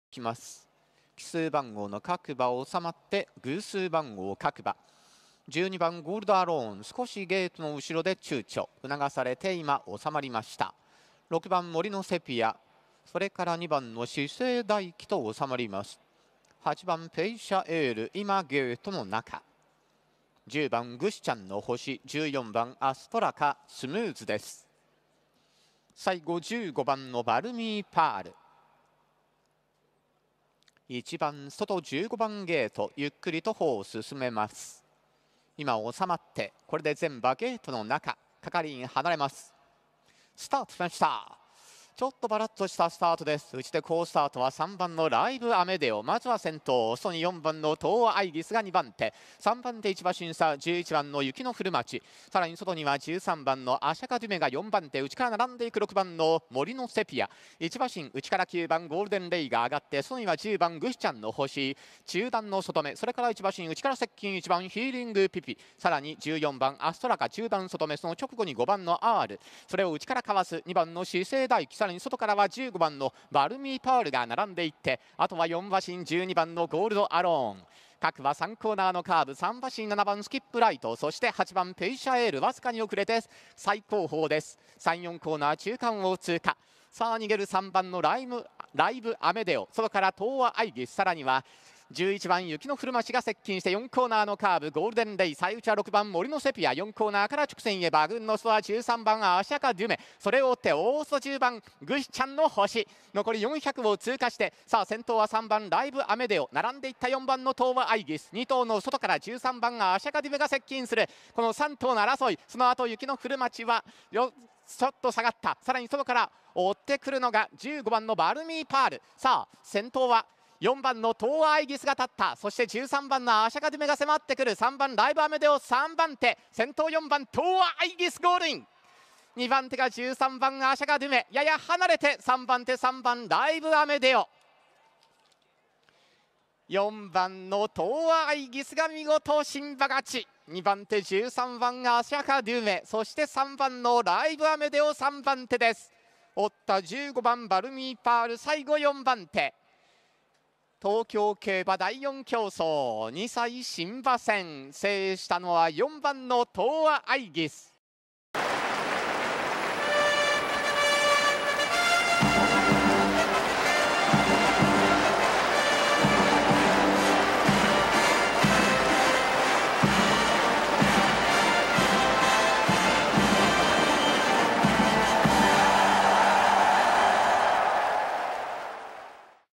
2歳新馬 ダ1400m (左) トーアアイギス 牝2 55.0 勝浦正樹 2023.10.22 出走馬：アシャカデュメ、ライヴアメデオ、バルミーパール、モリノセピア、アストラカ、シセイダイキ他
2歳新馬
11:25発走 / ダ1400m (左) / 天候:晴 / 馬場:良
4回 東京 7日目 サラ系２歳 新馬       (混)[指] 馬齢 15頭

トーアアイギス
生年月日 2021年3月30日
調教師 村田一誠 (美浦)
馬主 高山ランド
生産者 トーア牧場
産地 豊浦町
セリ取引価格 -
獲得賞金 720万円 (中央)
通算成績 1戦1勝 [1-0-0-0]
主な勝鞍 23'２歳新馬
近親馬 トーアイカヅチ、トーアオリンポス